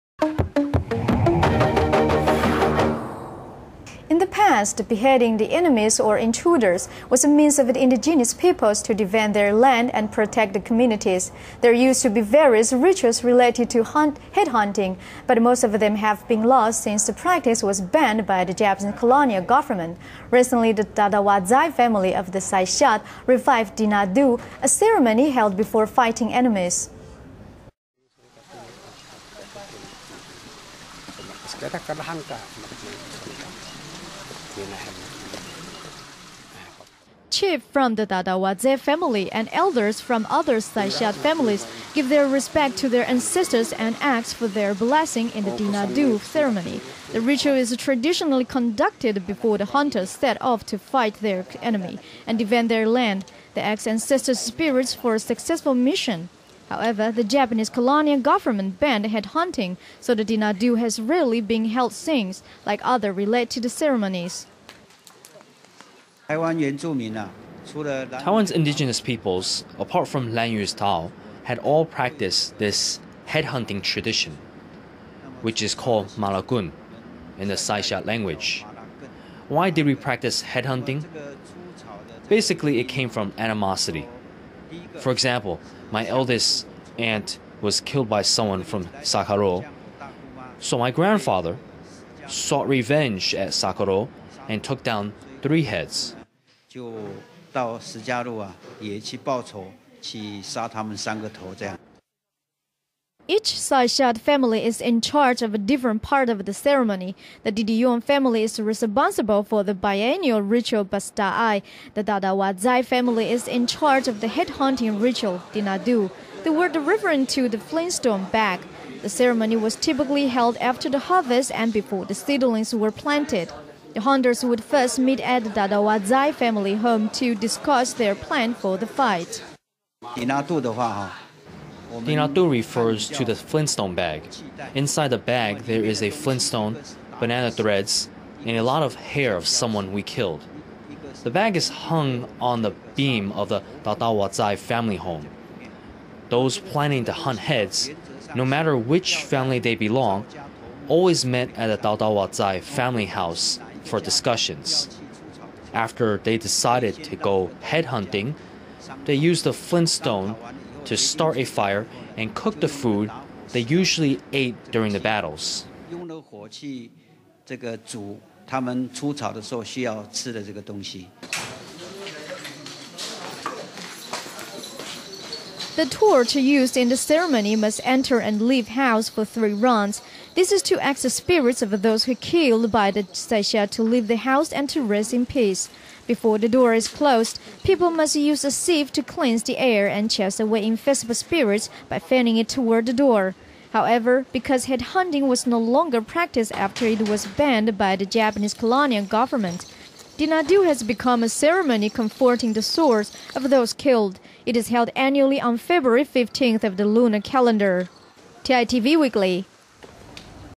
0320 #11 Saisiyat Tinato' Ceremony
In the past, beheading the enemies or intruders was a means of the indigenous people to defend their land and protect the communities. There used to be various rituals related to head-hunting, but most of them have been lost since the practice was banned by the Japanese colonial government. Recently the Tawtawazay family of the Saisiyat revived tinato', a ceremony held before fighting enemies.